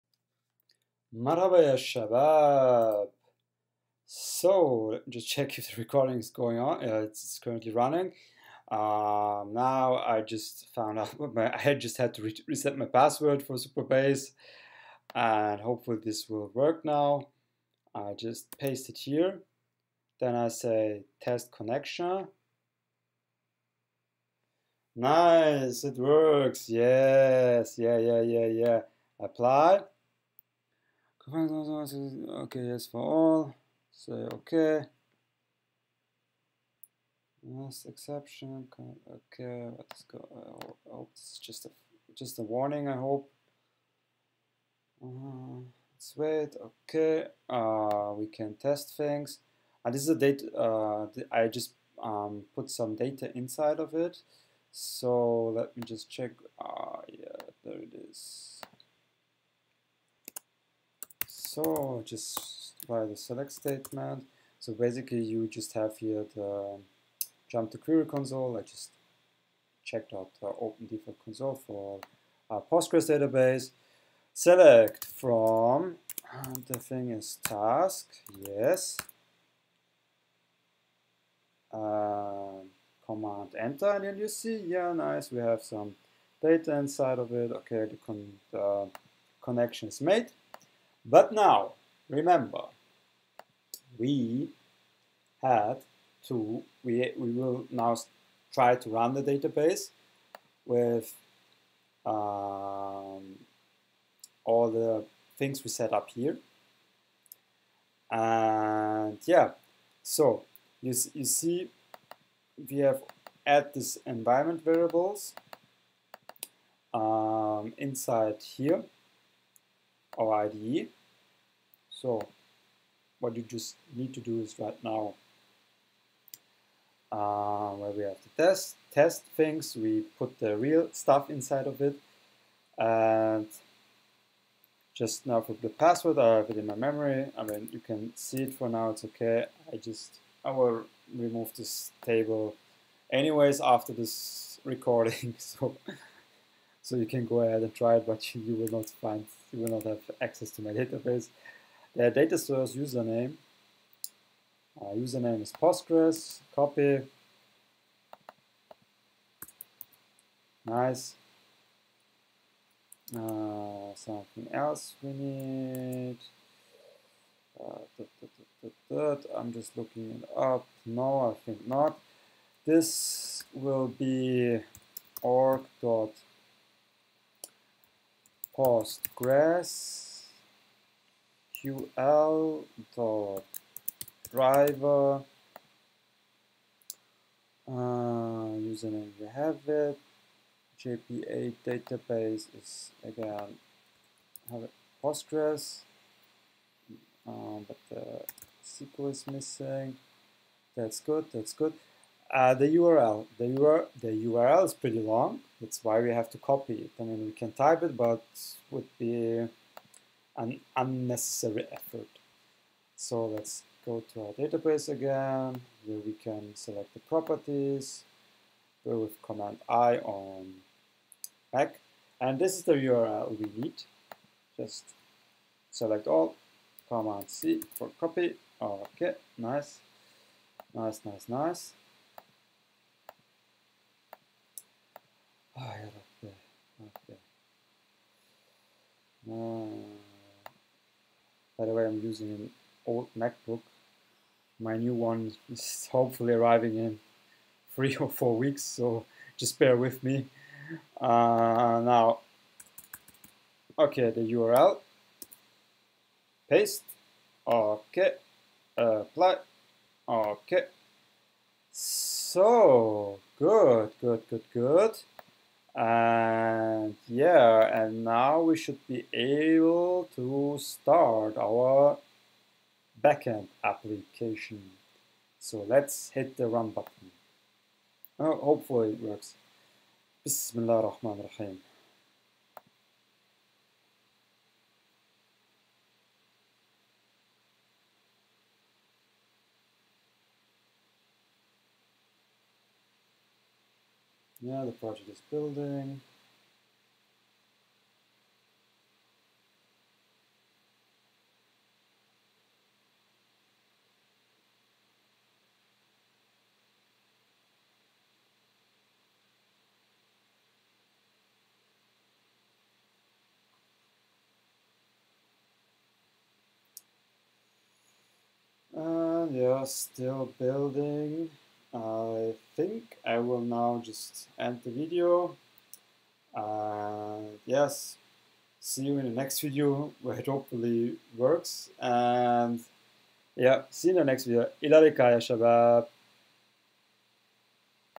Kotlin Spring Boot Tutorial - Part 13 | application.yml Secrets
In this Spring Boot backend tutorial, we successfully connect our application to a PostgreSQL database using Supabase. After resetting the Supabase password and setting up the database connection, we test the configuration in IntelliJ and confirm that the connection is live. You’ll see how to use the query console to run a simple SELECT * FROM task SQL command and verify that data is correctly retrieved from the Supabase PostgreSQL instance.

We also cover how to insert the correct JDBC URL, set the PostgreSQL driver, configure the datasource username and password, and ensure that the application.yml file includes all the necessary environment variables for a seamless connection. Whether you're running your backend on an old MacBook or prepping it for cloud deployment, this video will guide you through each configuration step.

This tutorial is perfect for Java developers working with Spring Boot, who want to integrate a PostgreSQL database using Supabase, configure YAML-based environment variables, test SQL queries inside IntelliJ, and prepare their backend for production deployment.

▬▬▬▬▬▬ Arabic to English vocab  ▬▬▬▬▬▬
hello guys and gals / marhaba ya shabaab / مرحبا يا شباب
bye / ila liqa / إِلَى اللِّقَاء

▬▬▬▬▬▬ L I N K S  to social media accounts   🔗  ▬▬▬▬▬▬